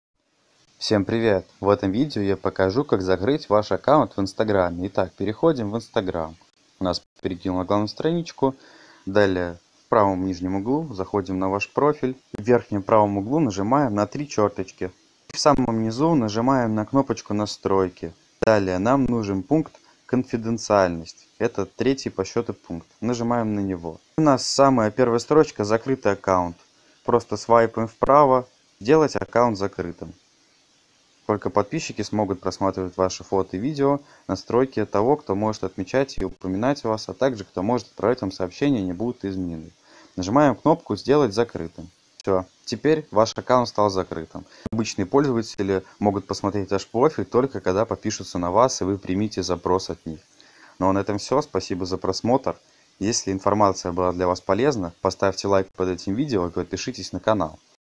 Как закрыть аккаунт в инстаграм
В этом видео я покажу как сделать закрытым профиль в инстаграм